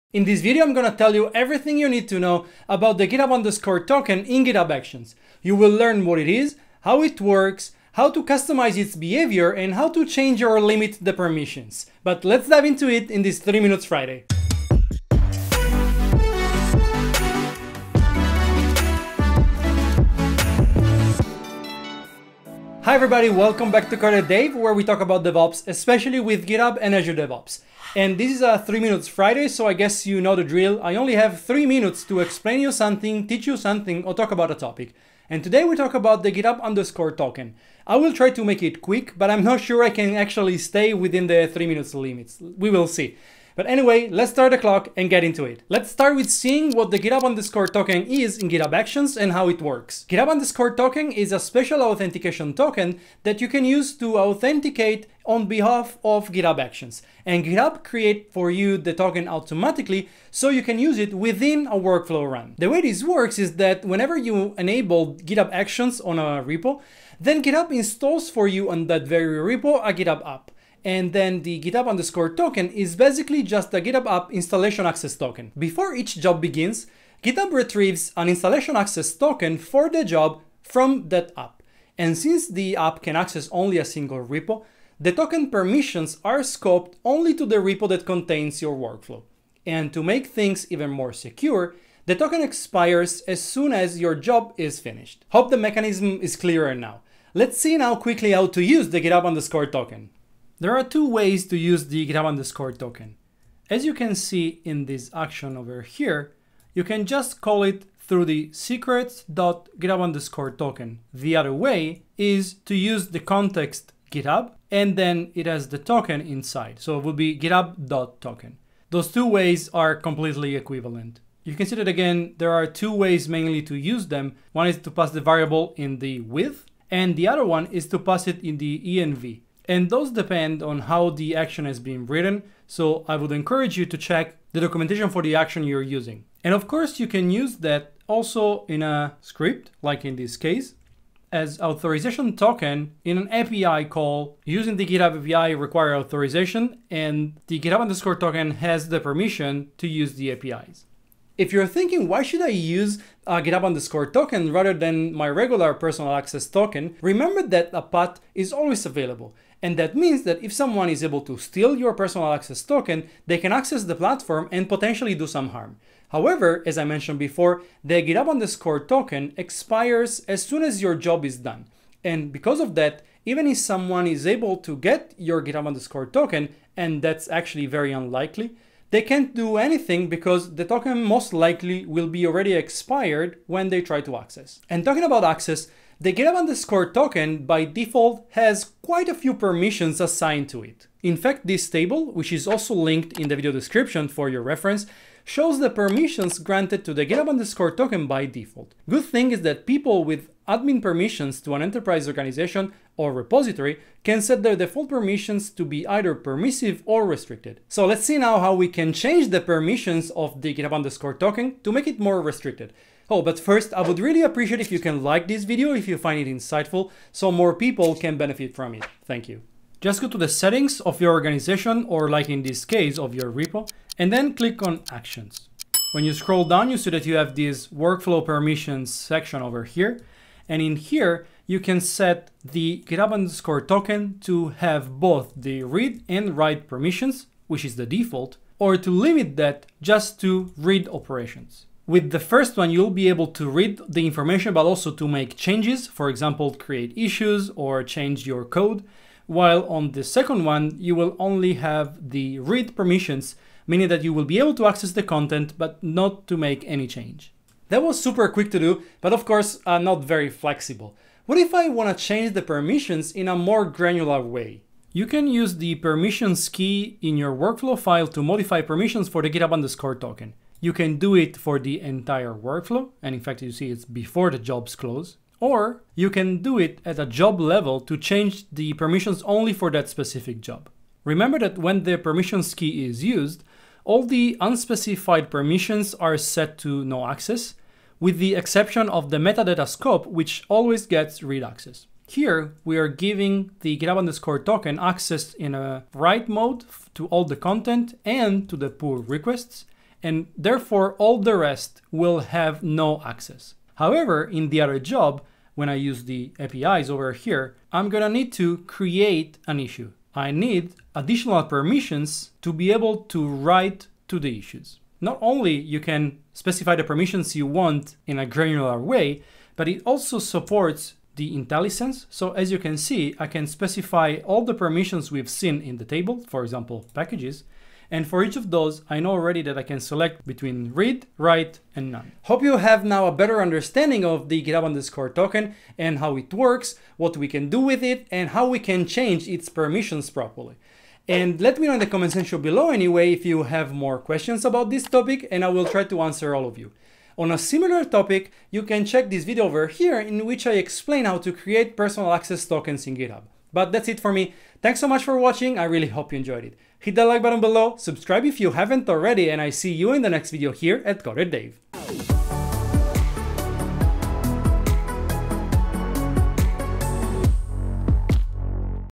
GitHub Actions: GITHUB_TOKEN Explained | How it works, Change Permissions, Customizations
In this video I'm gonna tell you everything about the GITHUB_TOKEN secret. You will learn how it works, how to customize its behavior and how to limit or change its permissions.

We will also see of course what is GITHUB_TOKEN and how the GitHub token authentication works.

🆘 NEED HELP? 🆘

⏲TIMESTAMPS
0:00 Intro
0:45 How GITHUB_TOKEN works
1:37 How to use GITHUB_TOKEN
2:31 GitHub.Token vs PAT
3:05 Permissions
4:17 Granular Permissions Assignment
5:32 Final Notes

❓QUESTIONS?